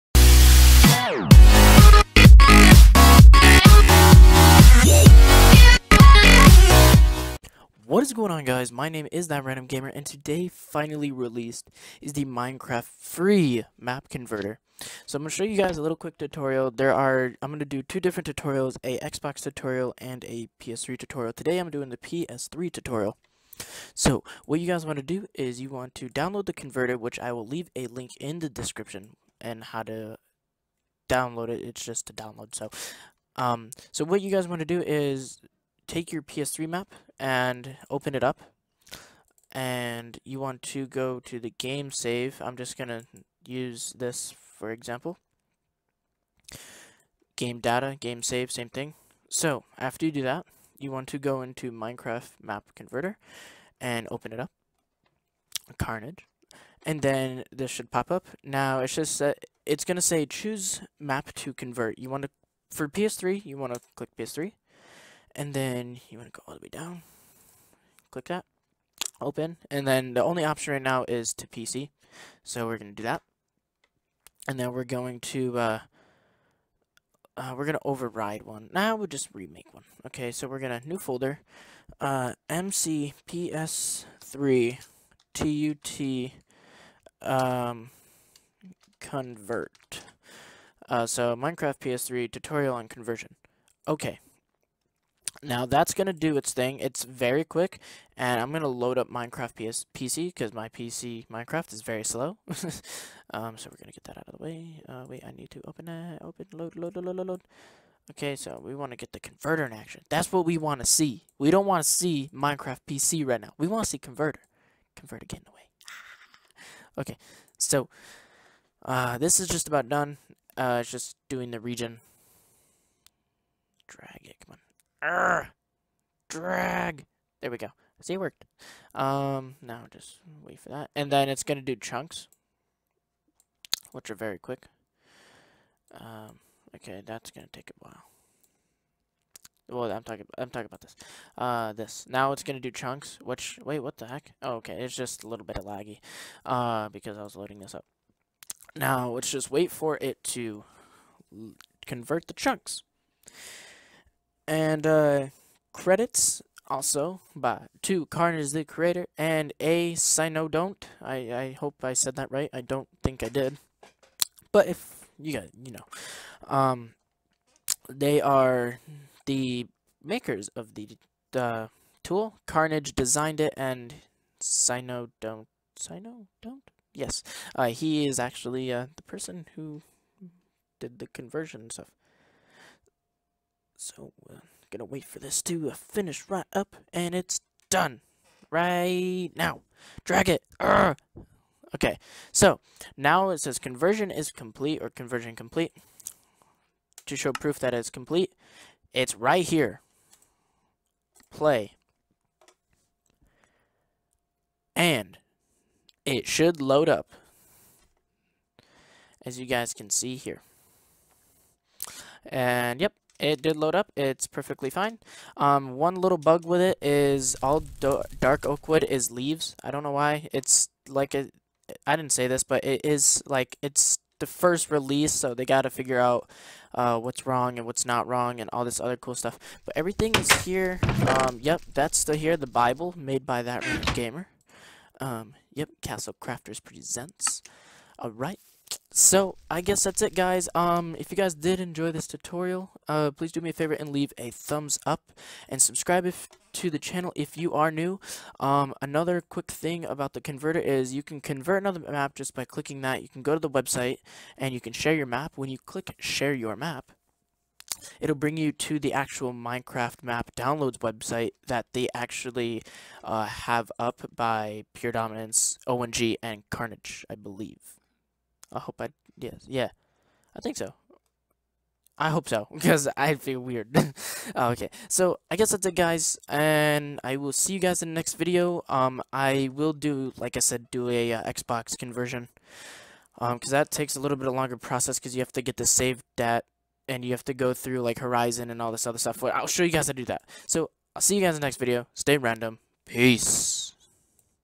Minecraft Map Converter Tool Tutorial (PS3 MAP)
► Minecraft Map Converter Tool Tutorial

Depending on which version of Windows you have, there might be a screen that pops up before it allows you to download it. If this happens for you, simply click "More Info" and then "Run Anyways".

Check Out The Makers Of The Converter

PSN- iThatRandomGamer

Stay Random!

* If You See This, Comment "CARNAGEROX"